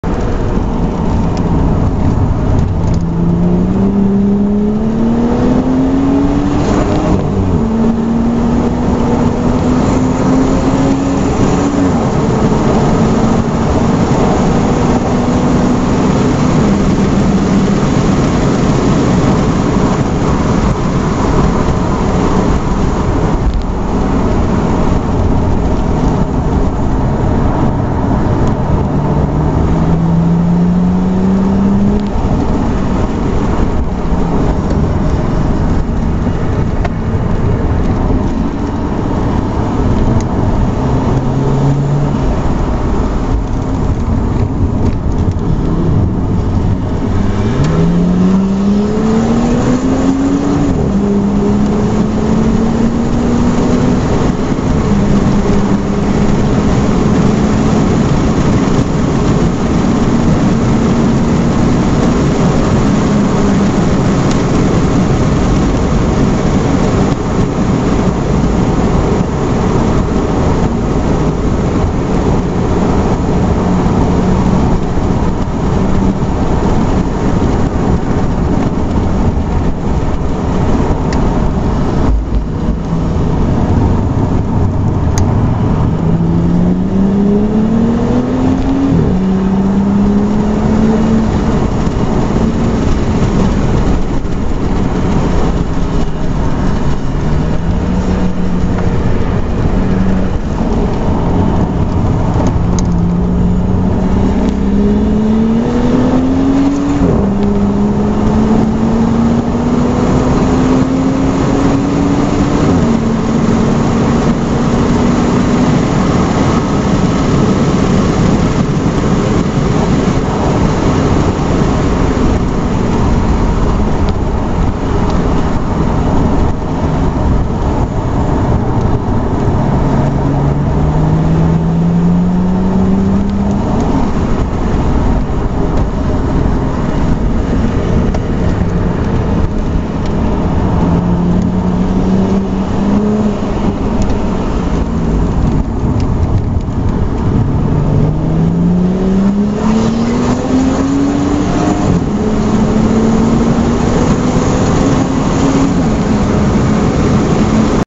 Ford FPV F6 310 Vs Porsche Sandown
FPV F6 runs down the Porsche Sandown Aug 2011